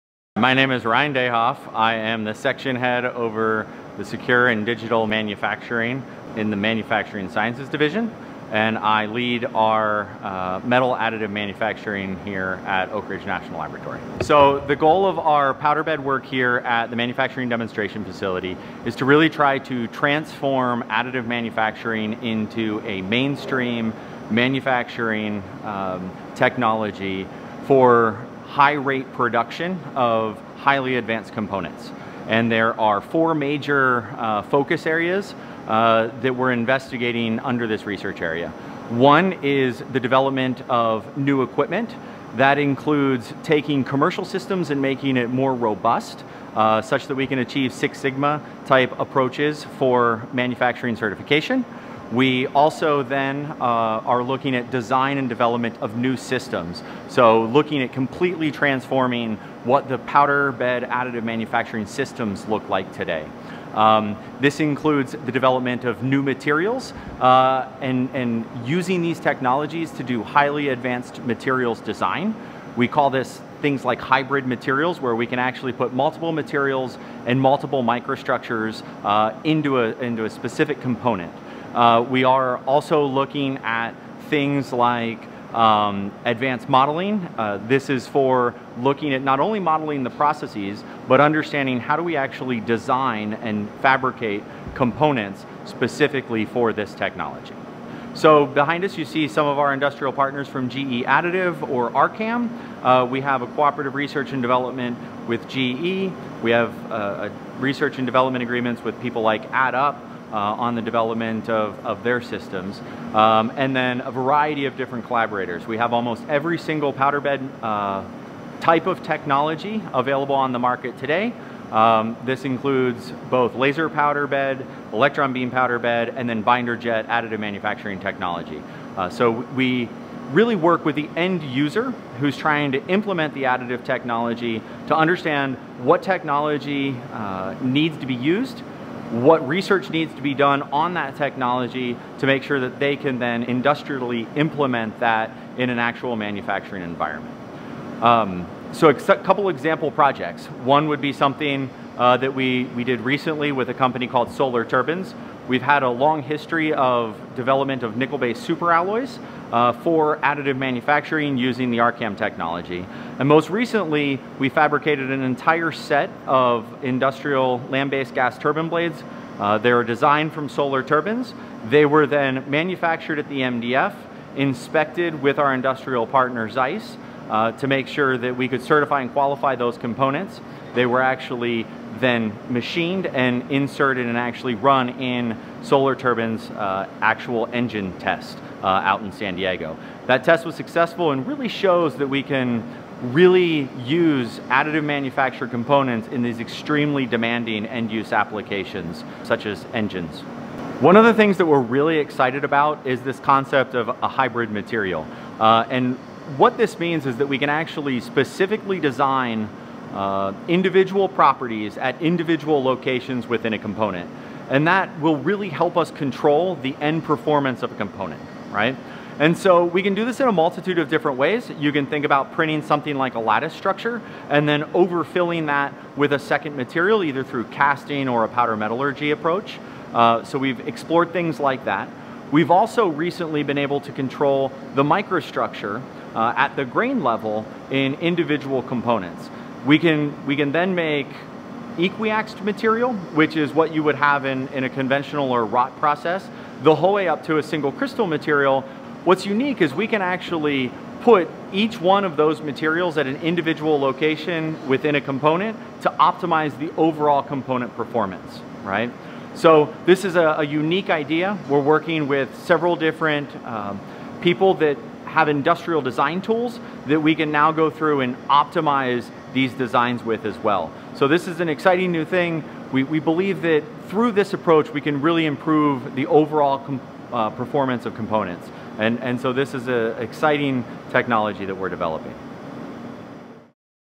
Powder Bed Manufacturing
Ryan Dehoff of Oak Ridge National Laboratory gives a brief overview of powder bed additive manufacturing capabilities at the Manufacturing Demonstration Facility.